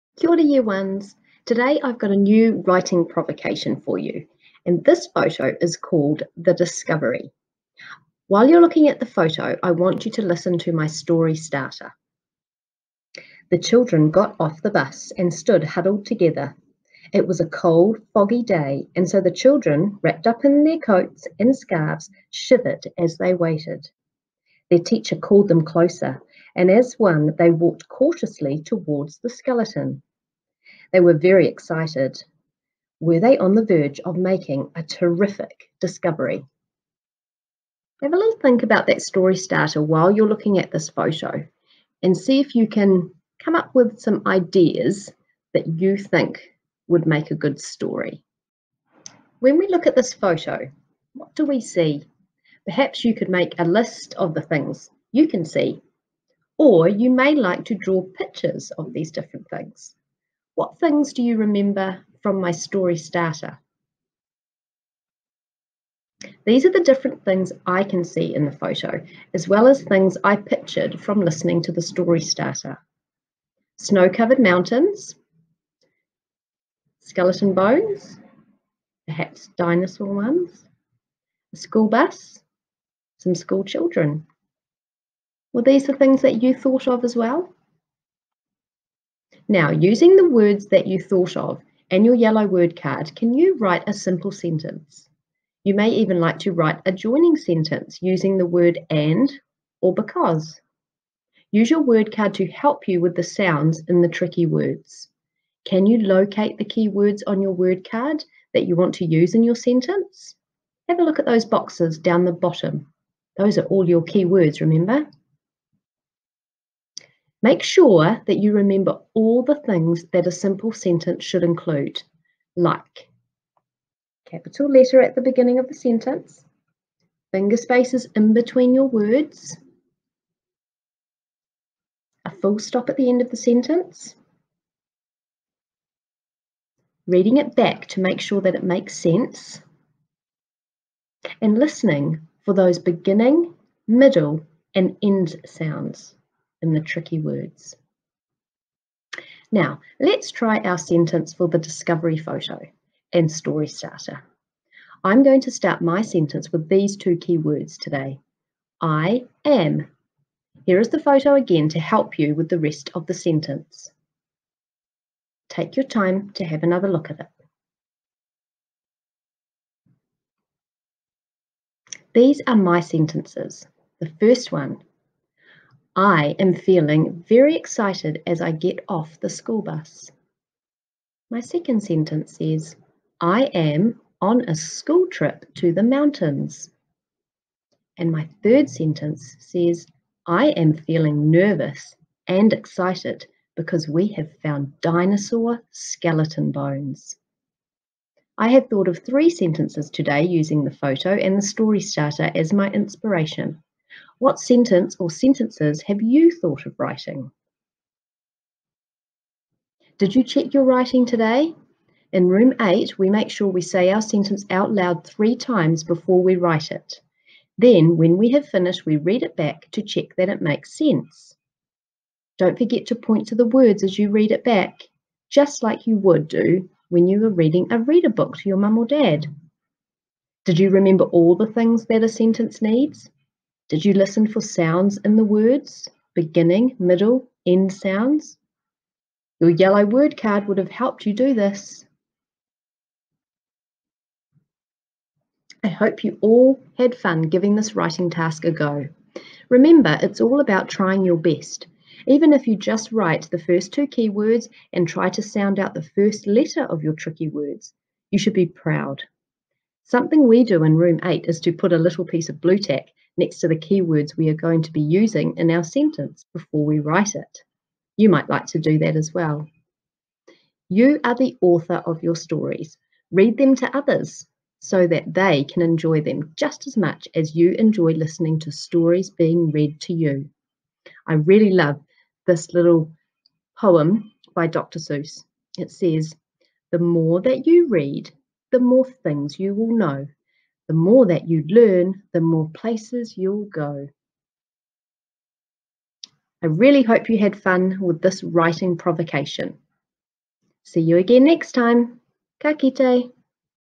Writing with Ms Smith (The Discovery)
Follow along as Ms Smith writes a sentence about the picture. Use the picture and story starter as inspiration for your writing.